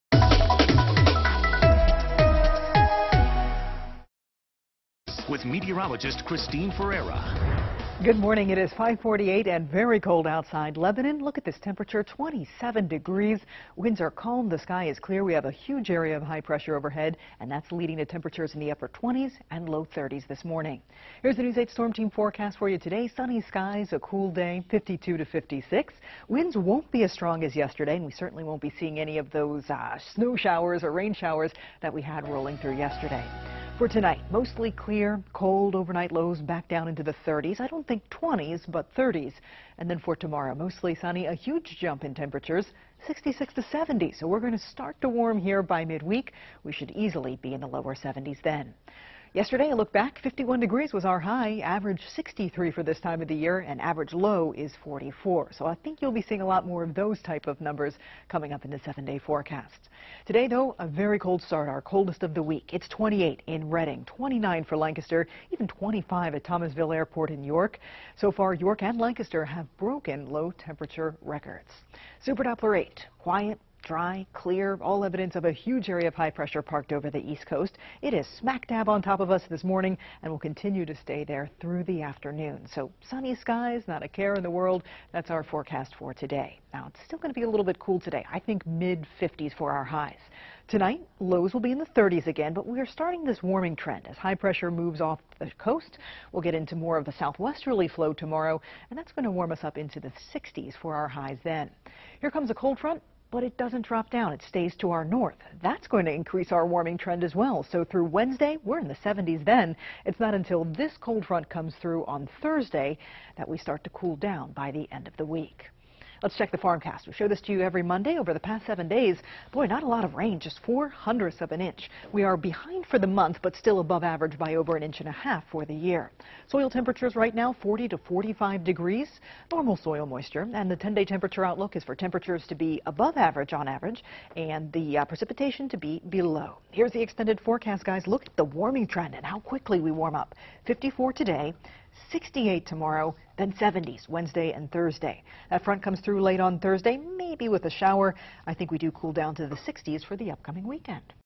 Sunny and cool today
Sunny skies and cool today, but a warm up is on the way. Check out Christine's forecast!